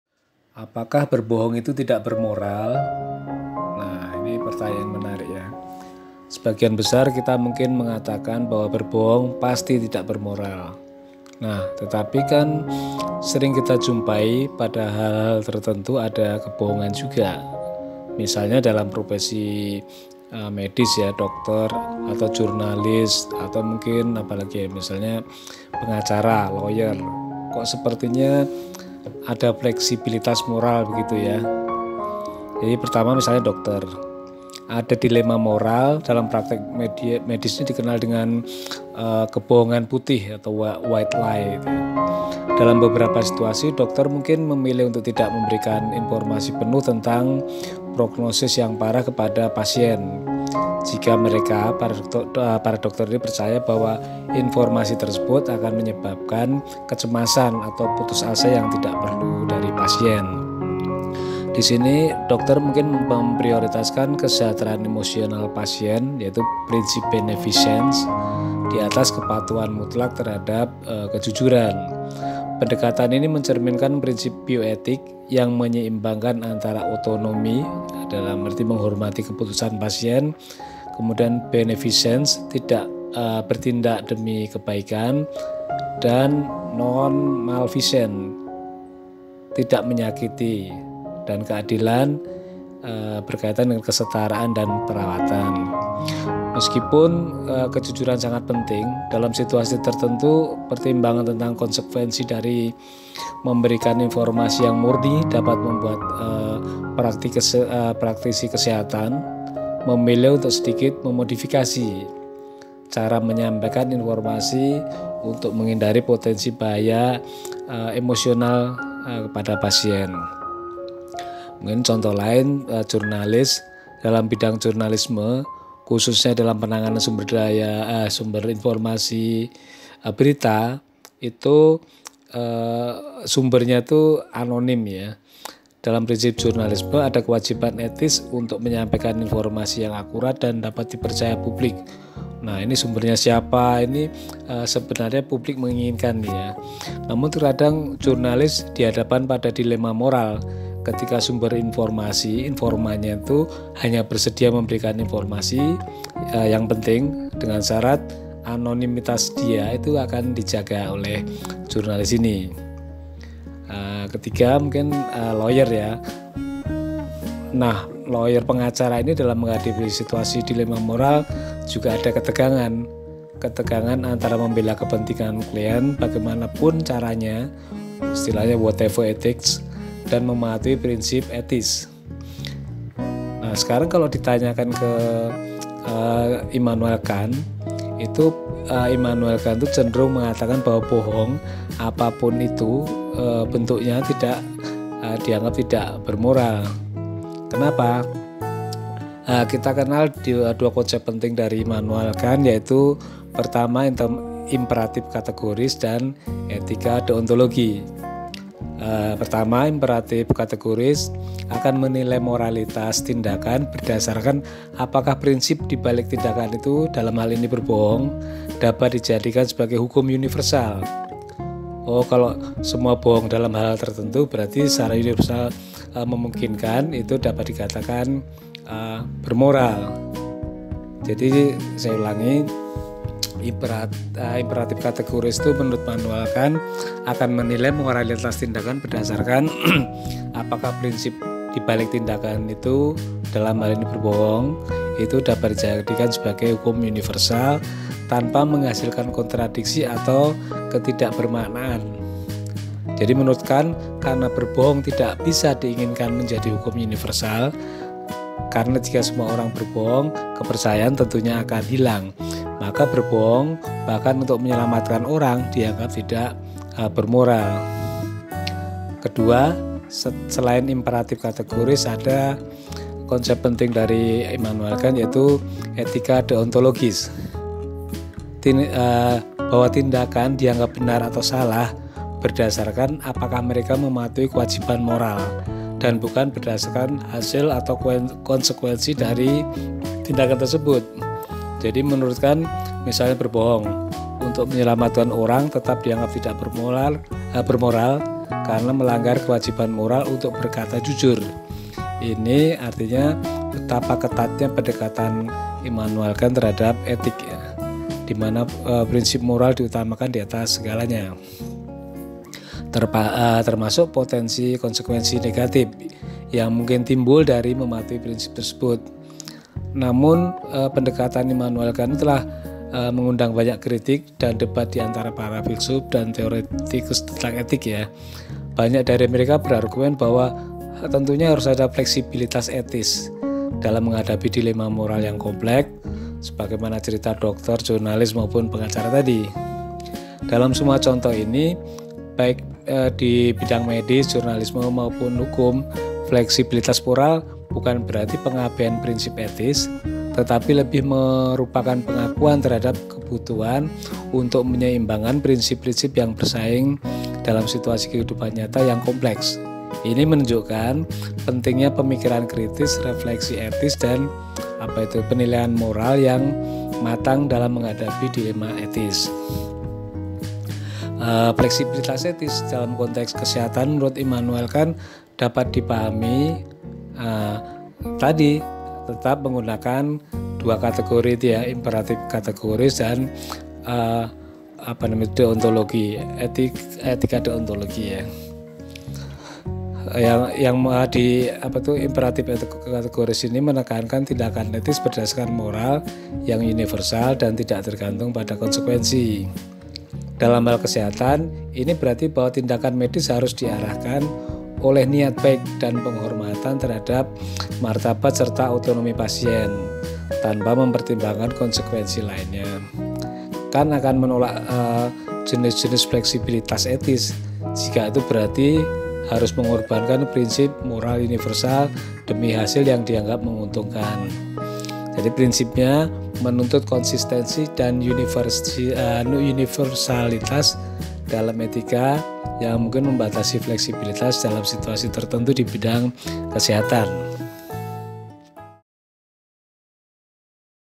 Apakah bohong itu tidak bermoral?
Dalam esai "On a Supposed Right to Lie from Philanthropic Concerns," yang ditulis oleh Immanuel Kant pada tahun 1797, Kant menanggapi sebuah dilema moral yang dihadirkan oleh kritikus filsafat Prancis, Benjamin Constant. Singkat cerita, dilema tersebut berkisar pada pertanyaan apakah dibenarkan berbohong untuk melindungi orang yang tidak bersalah dari pembunuh yang mengejar mereka.